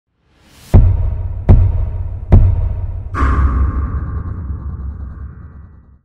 [싱테나] 2019 싱글즈 코스메틱 어워드 BEST DERMA 바이오더마
2019 싱글즈 코스메틱 어워드 BEST DERMA는 바이오더마 아토덤 크림입니다!